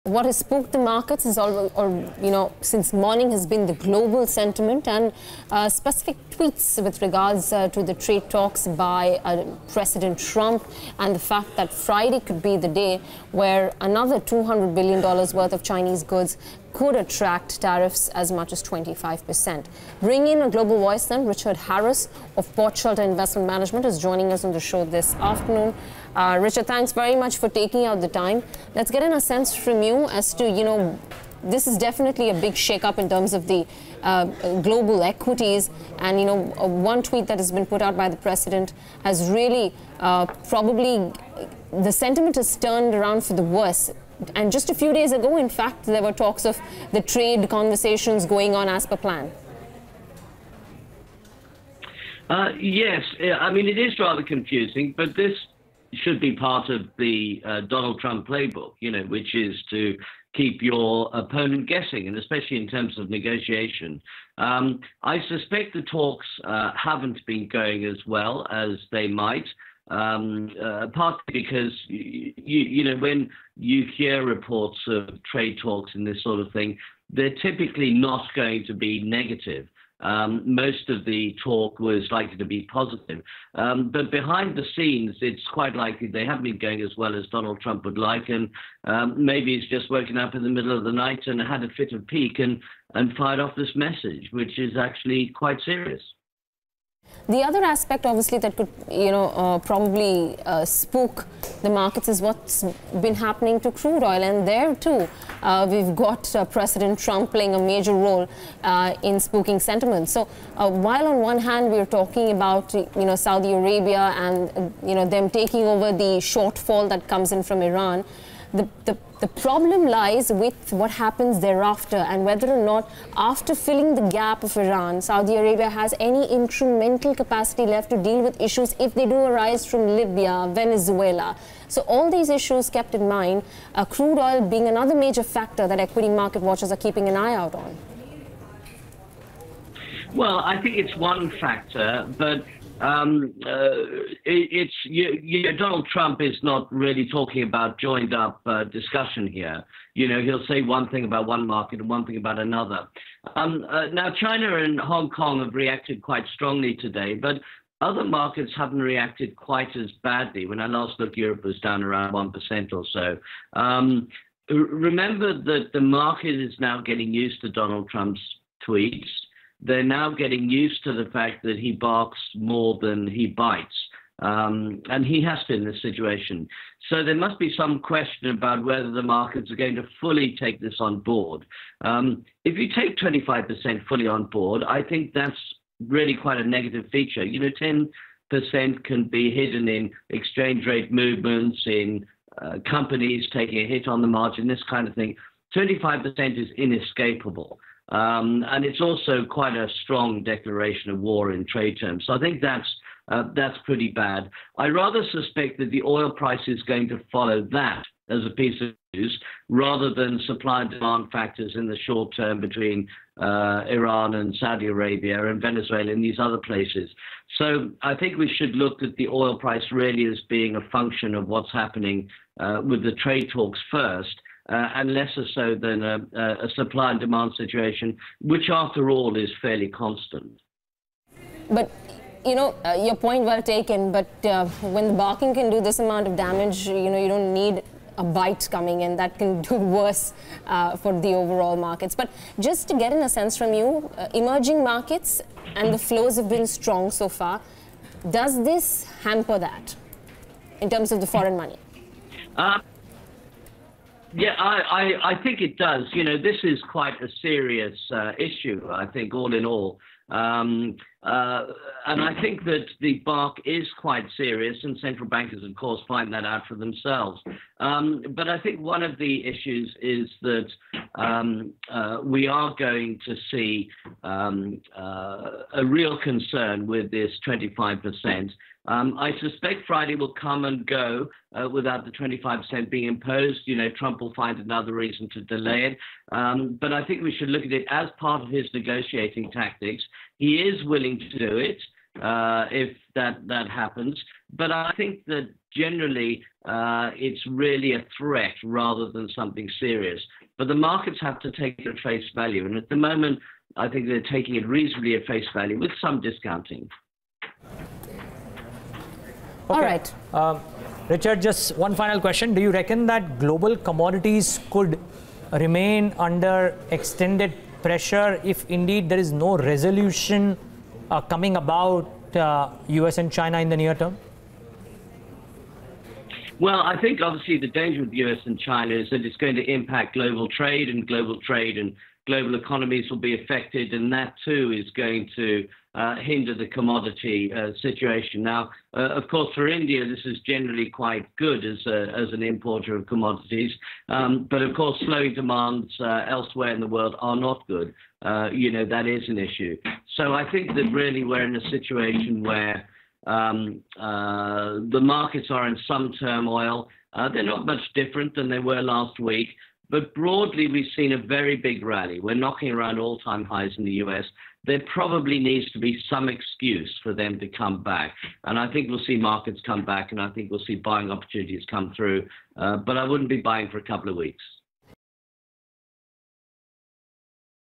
Richard Harris: Trump Will Find Another Reason To Delay Trade Talks
A discussion with Richard Harris of Portshelter on how impactful is the resumption of trade tensions.